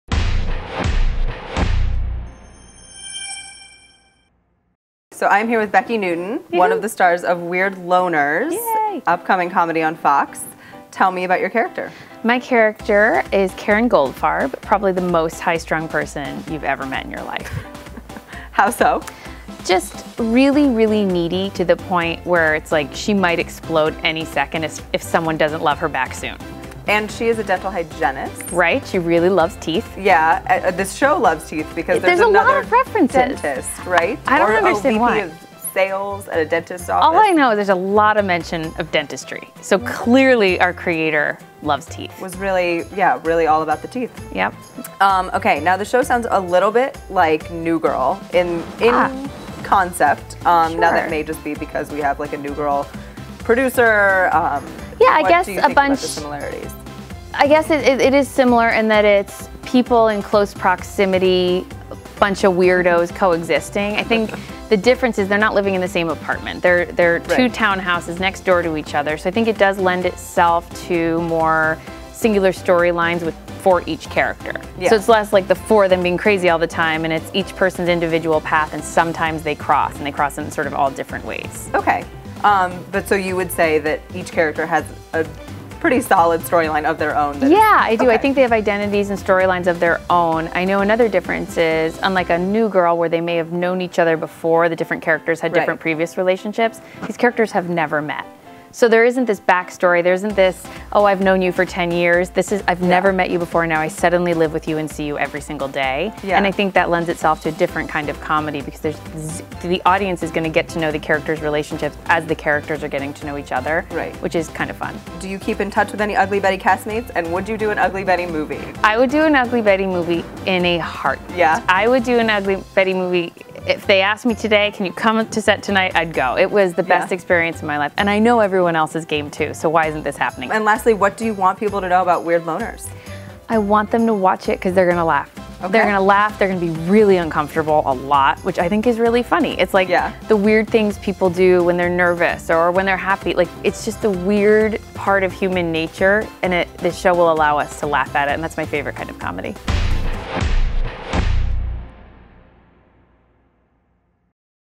‘Weird Loners’ Star Becki Newton on ‘New Girl’ Comparisons and ‘Ugly Betty’ Reunion
Actress tells TheWrap she “would do an ‘Ugly Betty’ movie in a heartbeat”